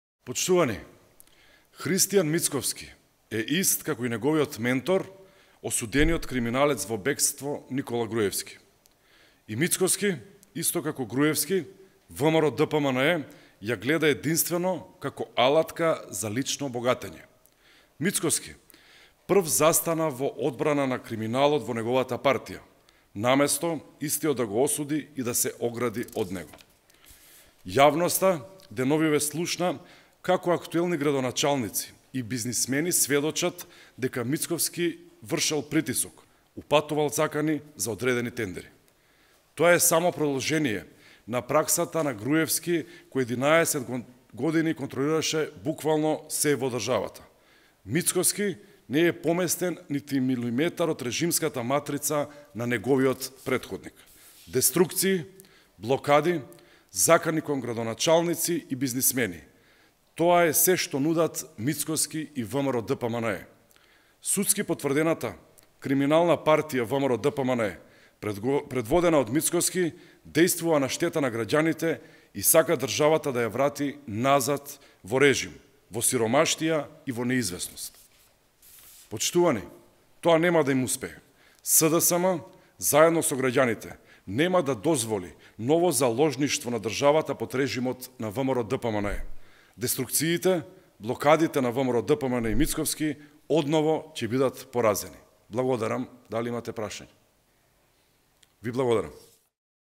Мицкоски е ист како Груевски, на браникот на криминалот, заканите и притисоците
Прес-конференција на Мартин Костовски, пратеник на СДСМ